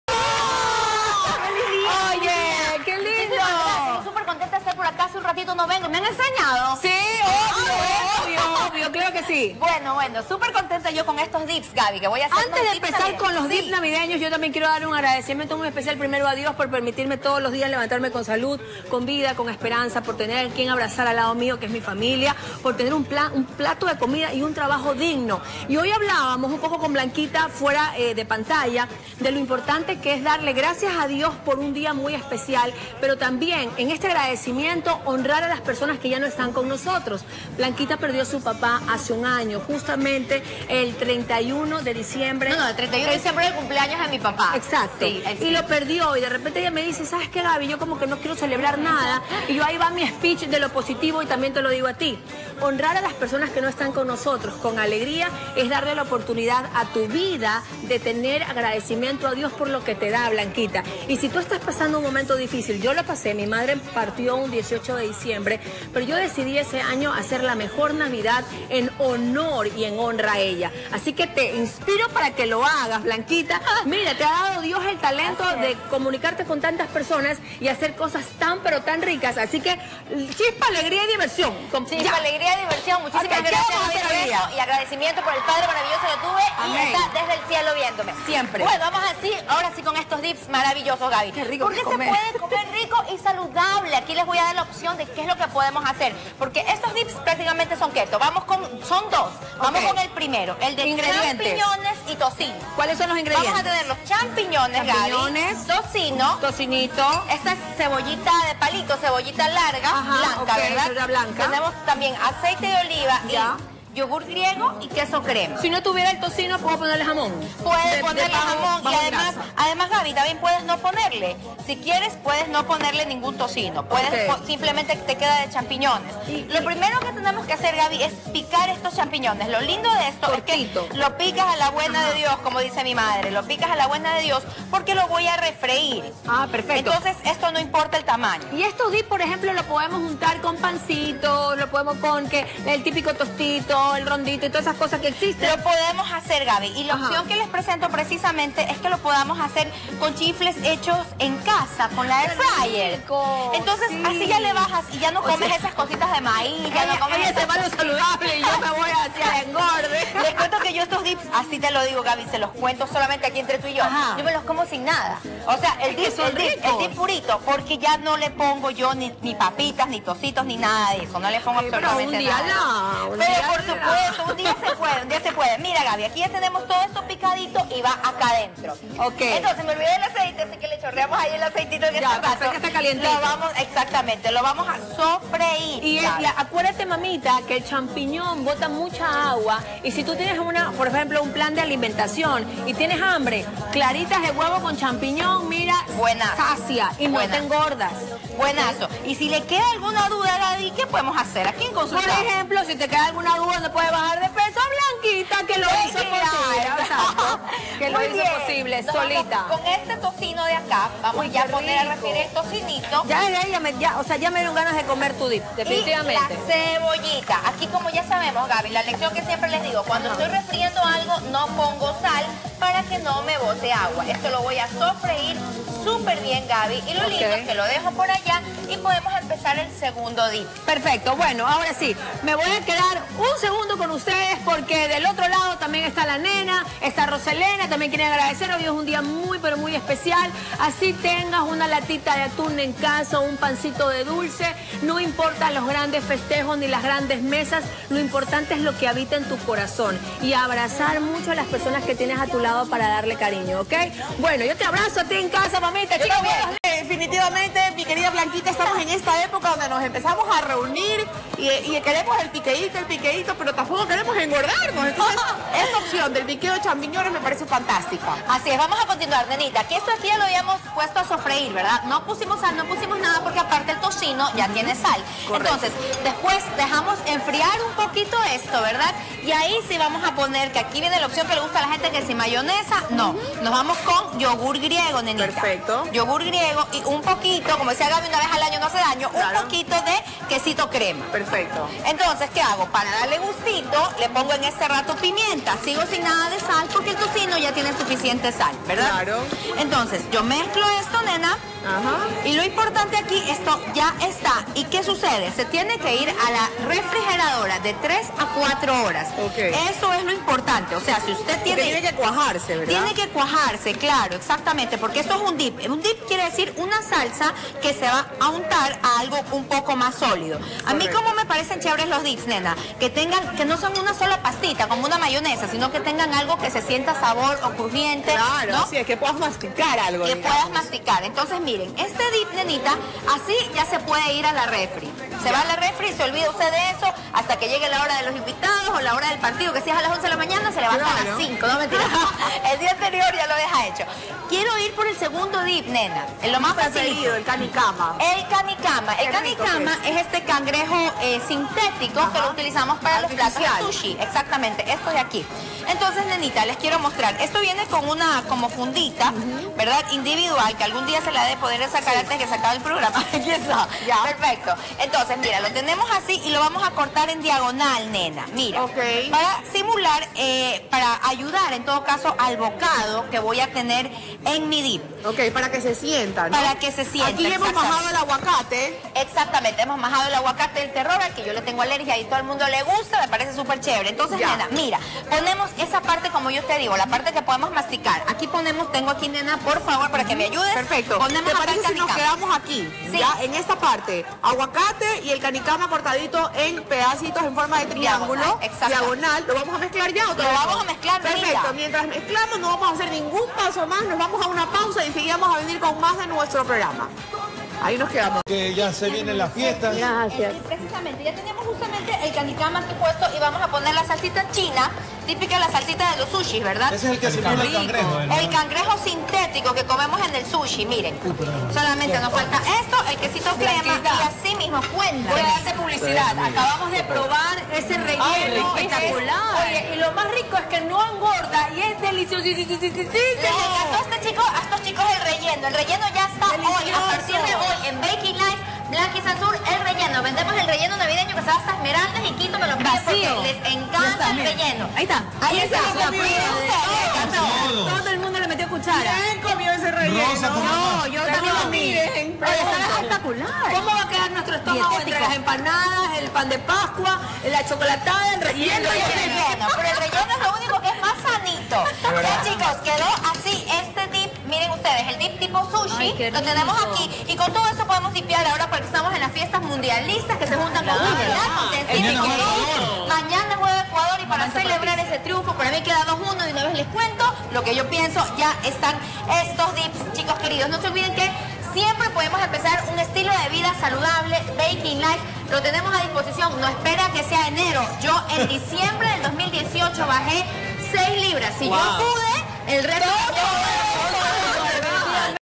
Receta del día: dips de champiñones y tocino
En el segmento de cocina, la chef Blanca Sánchez nos enseña cómo preparar dips de champiñones y tocino. Toma nota para innovar en la cena de navidad.